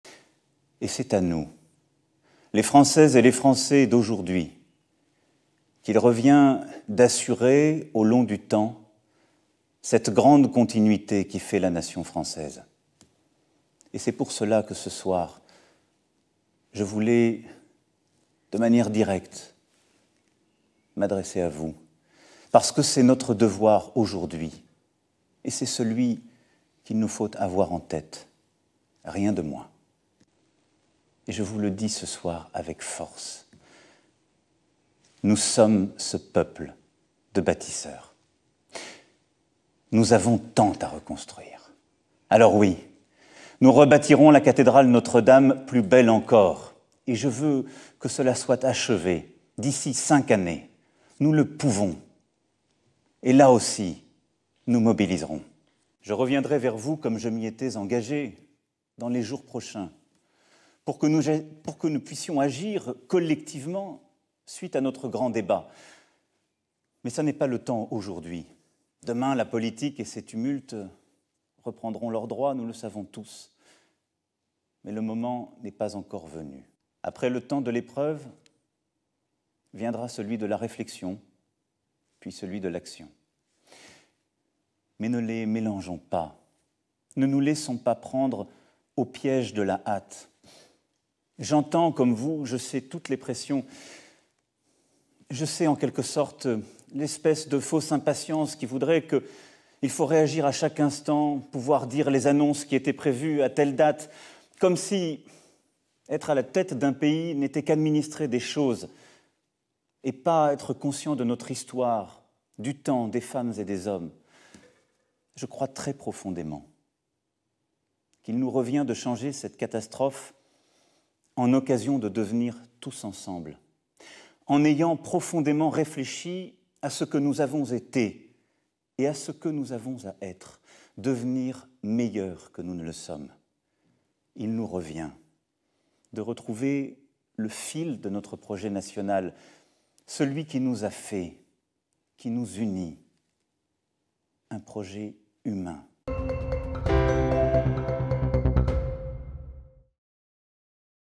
Emmanuel Macron veut rebâtir la cathédrale Notre-Dame de Paris d'ici à 5 ans
Dans son allocution télévisée, Emmanuel Macron est revenu mardi soir avec émotion sur l'incendie de la cathédrale Notre-Dame de Paris rendant un hommage prononcé aux pompiers. Il a notamment indiqué vouloir rebâtir l'édifice d'ici à 5 ans.